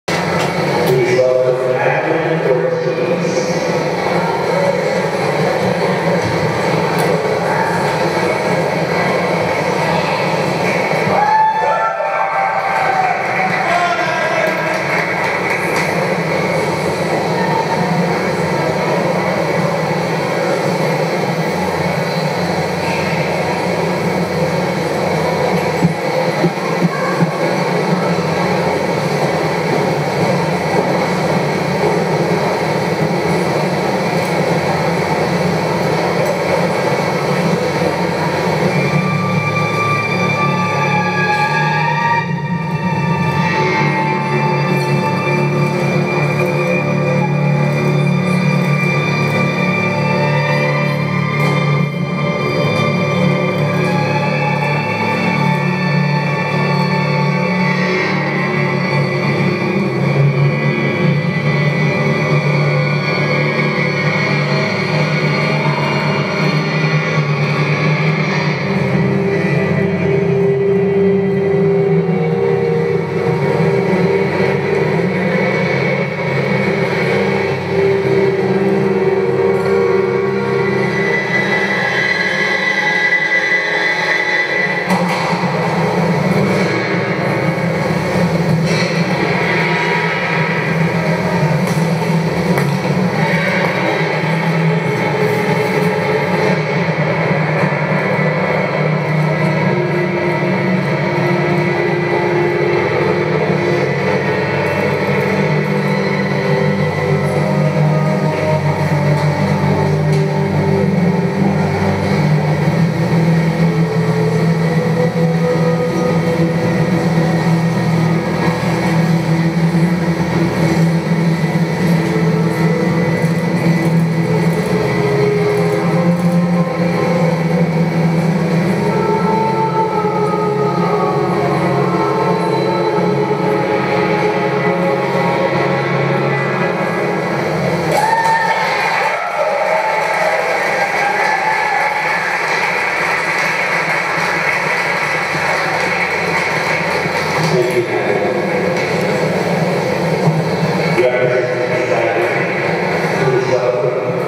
2014 June ISI Competition - Madeline Latessa Ortiz - Artistic FS 4
Madeline won this group and then went on to win the Artistic Run-Off with this program, choreographed by Greg Wittrock.